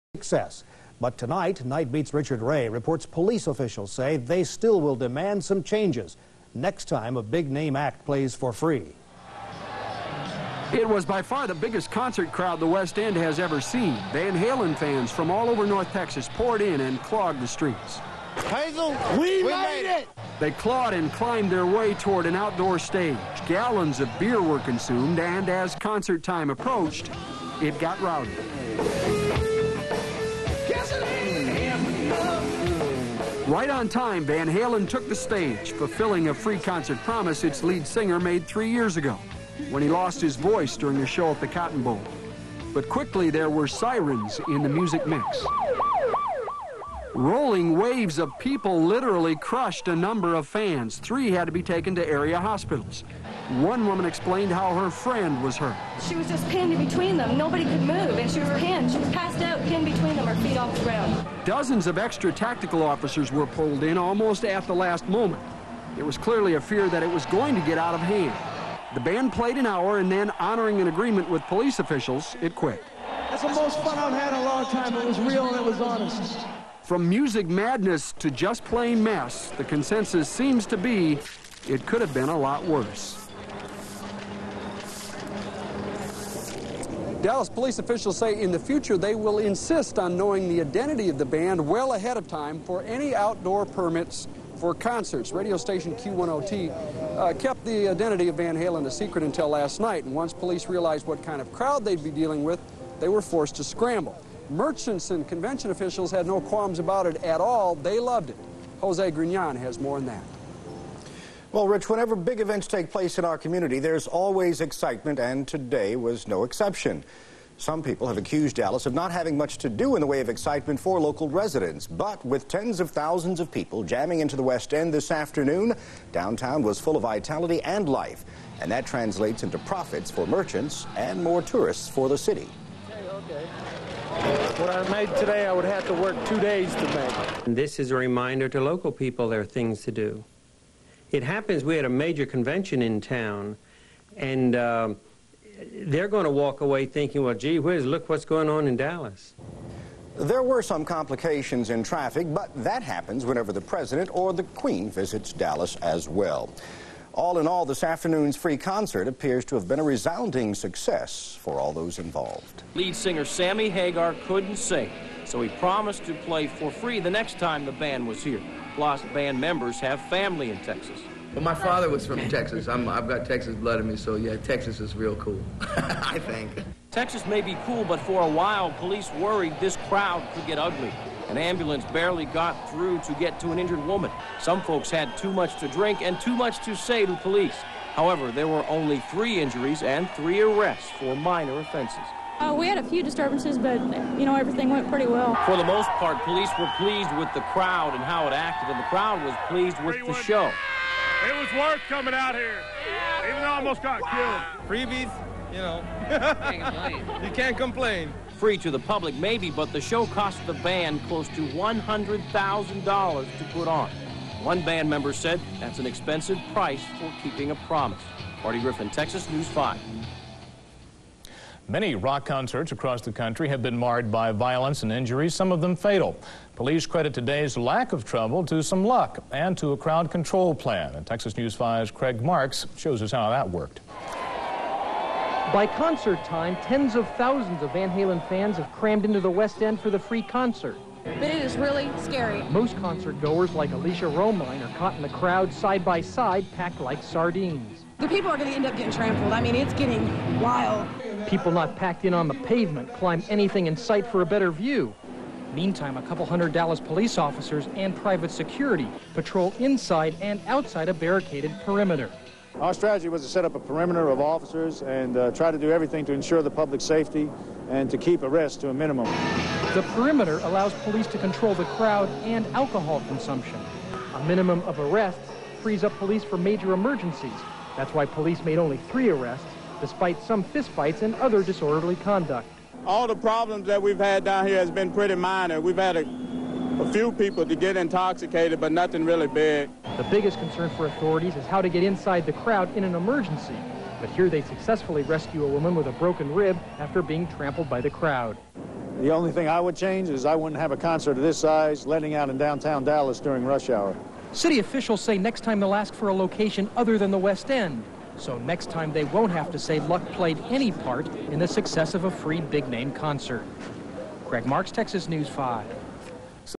Van Halen Free Concert
So there was hype all day on the radio and when they announced it, I bolted from work (suddenly forgot about an appointment) and made it from McKinney all the way down to the West End market place.   I tried to get up to the front but I was being squashed and suddenly couldn't move.  It could have been easy to panic but some how made it to the side where there was some breathing room.
It was a great show, they only played an hour because of too many complaints but it was a cool thing.